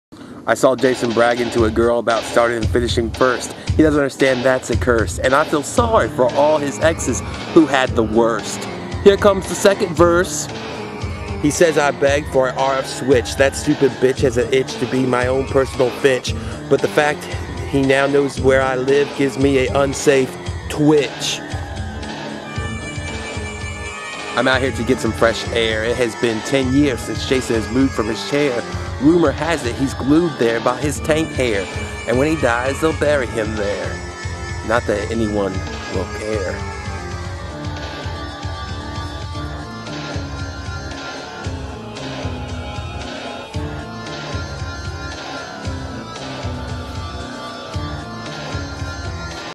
Rap Attack Back On Jason
As promised for my 700th sub. Timmo said I needed a beat, so I dug something up. All in good fun as always.